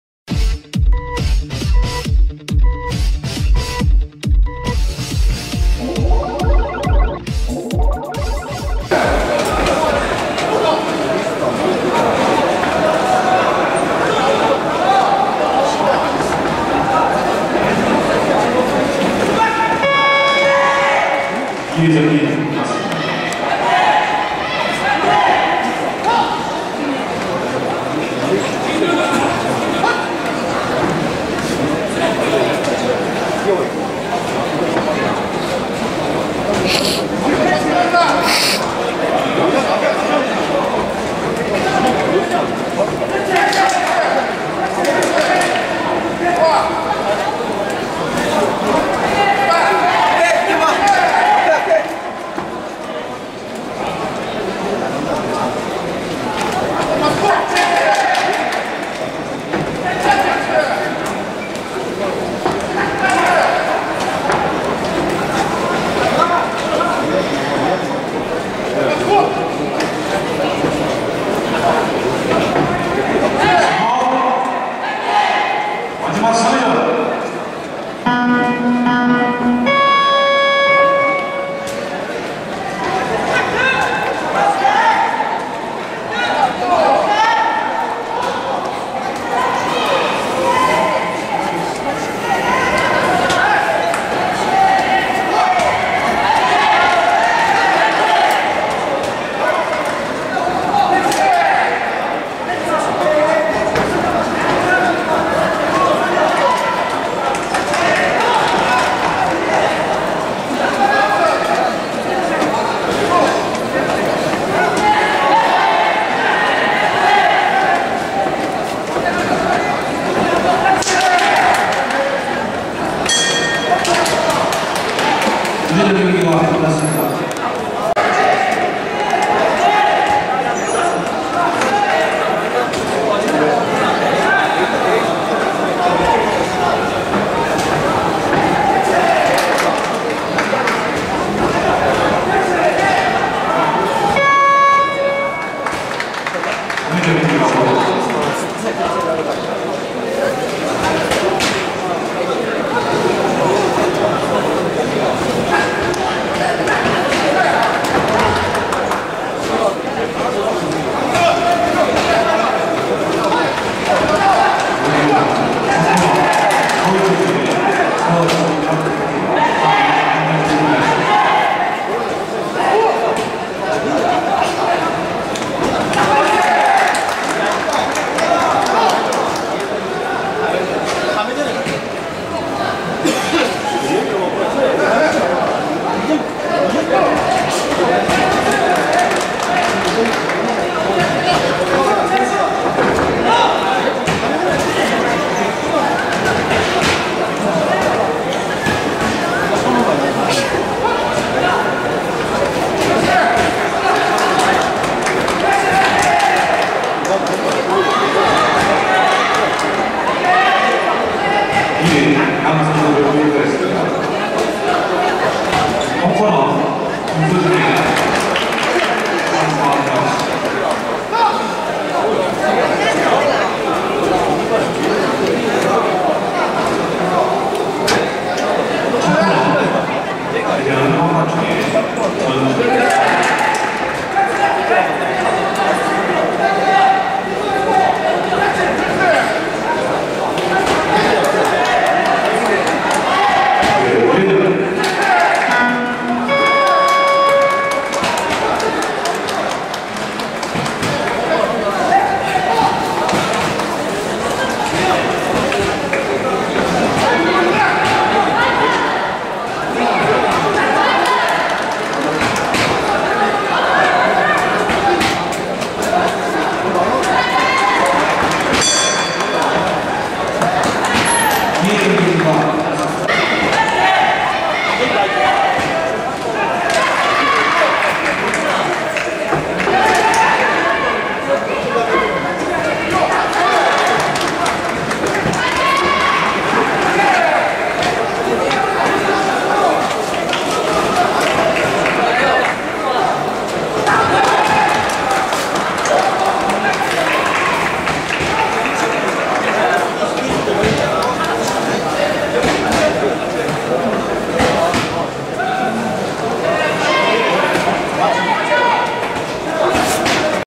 (요청경기)제주출신 형제복서 兄강경돈 弟강경석 동생 강경석선수
2009년도 제20회 대한복싱협회장배 -54kg급
홍코너 강경석(제주남녕고)
청코너 장이운(충남체육고)